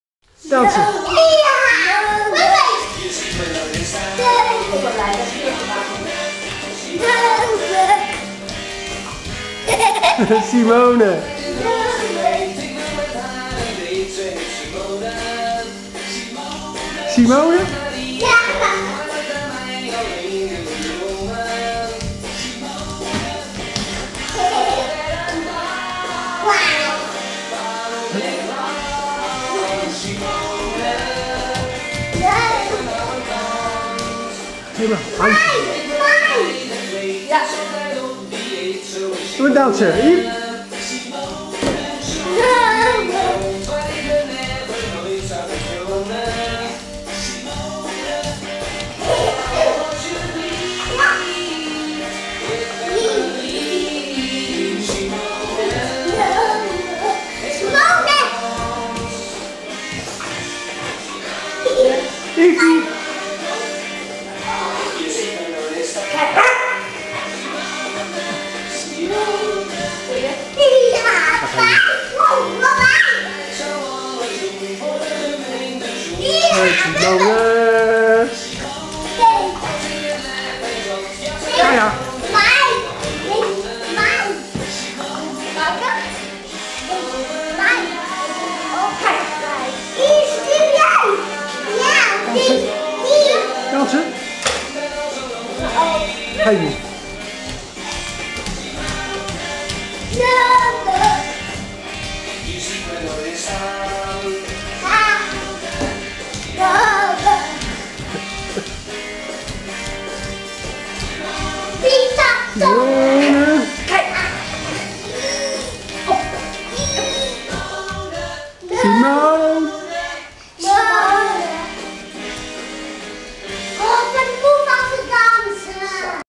Matthis en Yves zijn fan van the KIK
Matthis en Yves dansen op de muziek van the KIK. Simone.